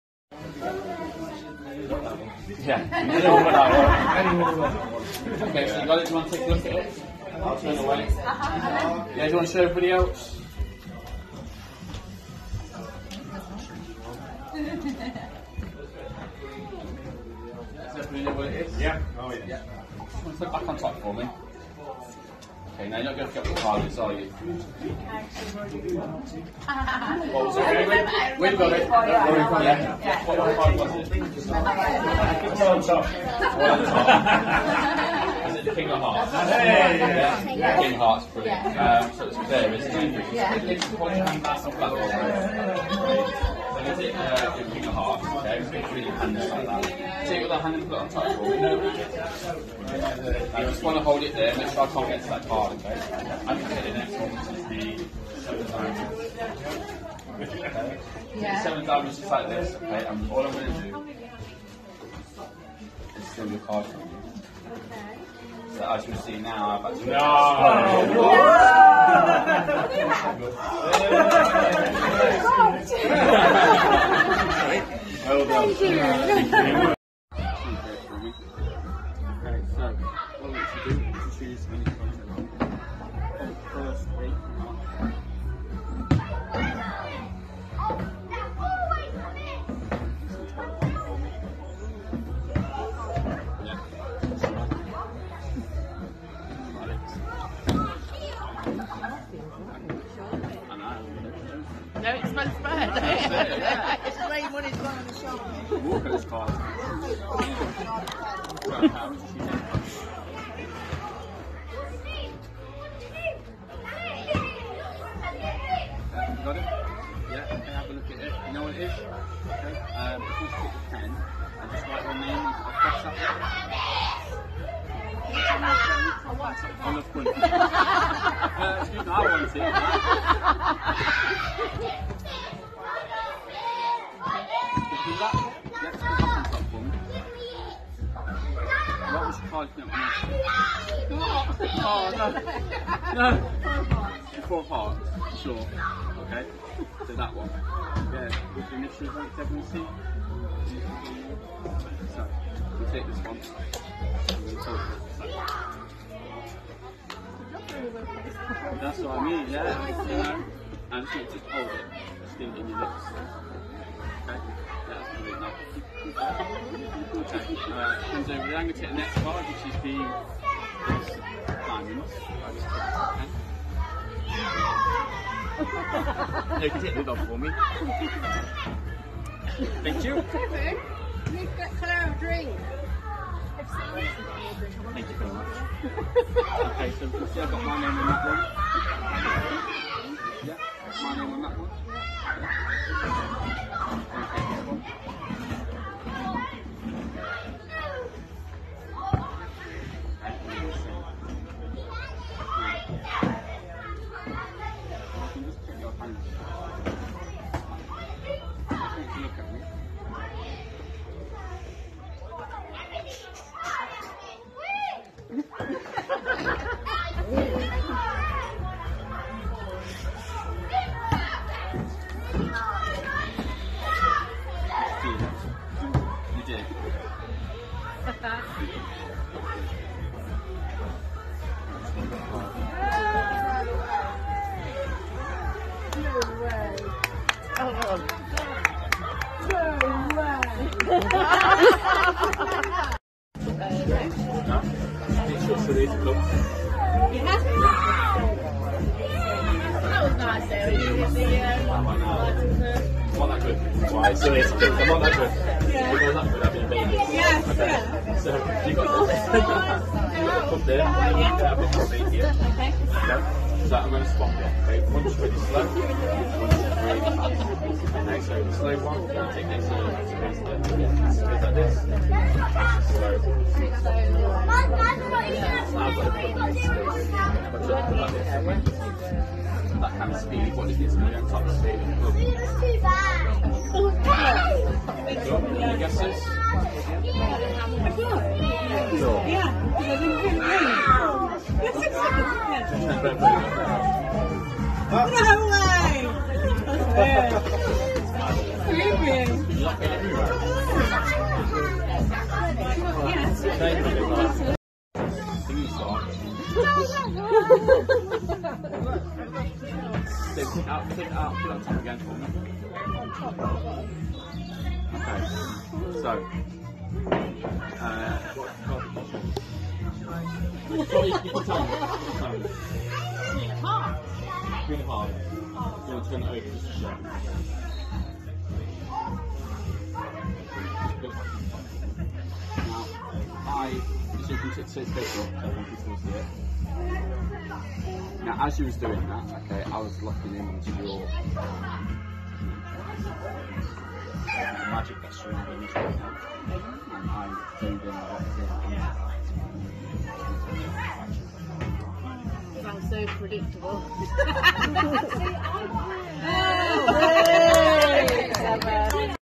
Sycamore Magic Performance Video 20/05/2023
A small video collection of some magic performed at birthday party